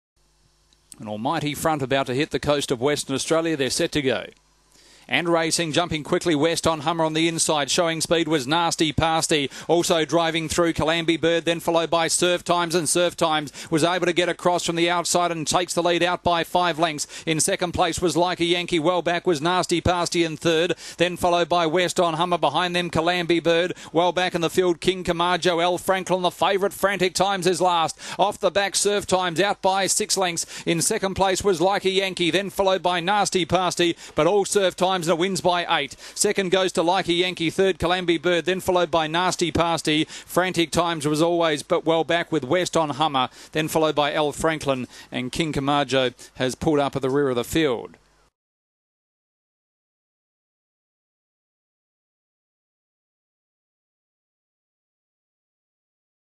Surf Times 20May09
Surf Time the 3rd of the Surf Lorian  litter to win it maiden at cannington in slick 30.86